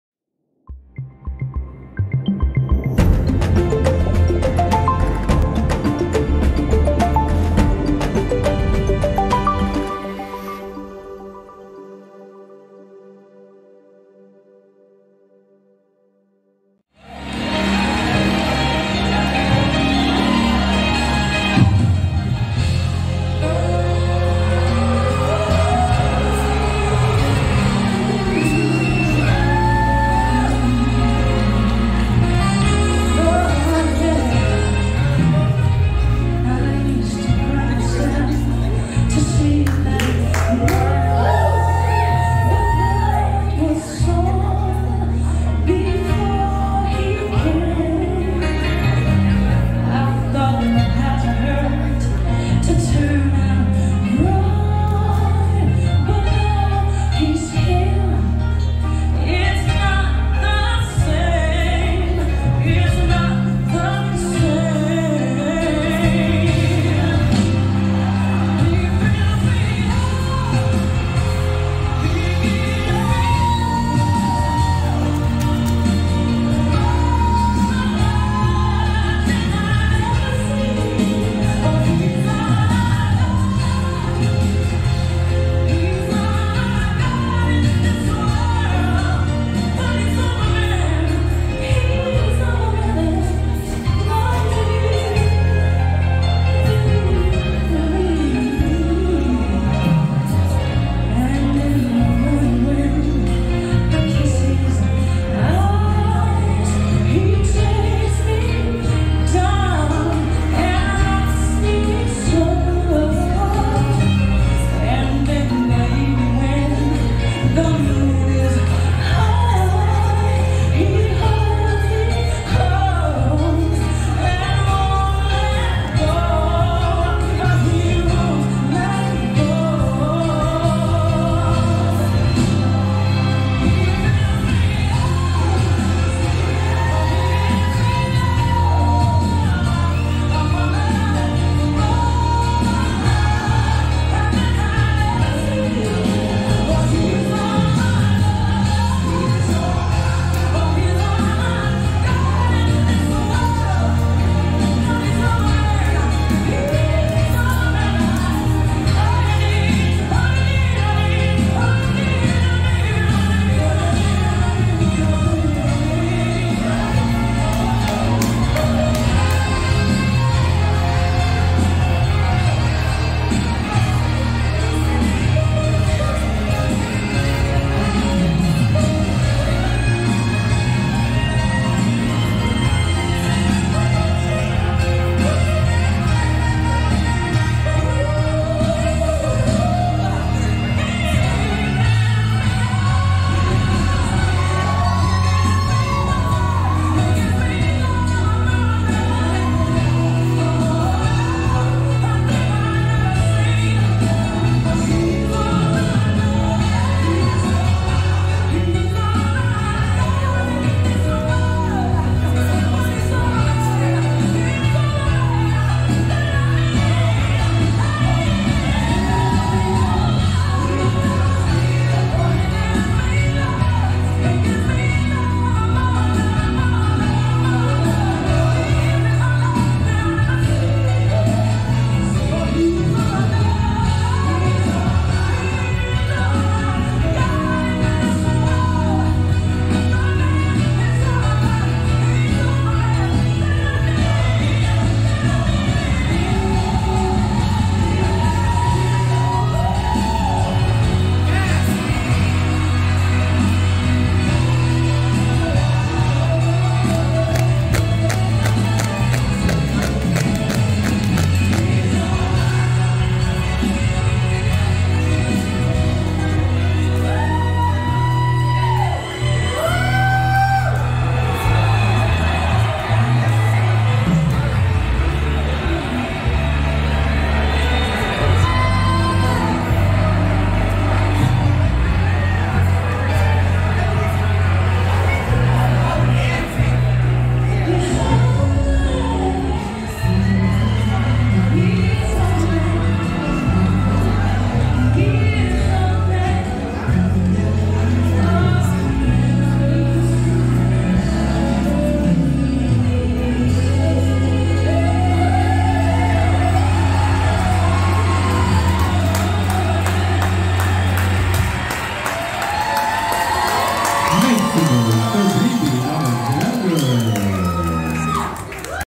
Knockout (Round 4): Drag Ballads - BeeBee Alexander
The dancing diva of Knoxville, BeeBee Alexander.
In the Drag Ballads Category, contestants were asked to show emotion through iconic ballads.

I claim no rights to the music.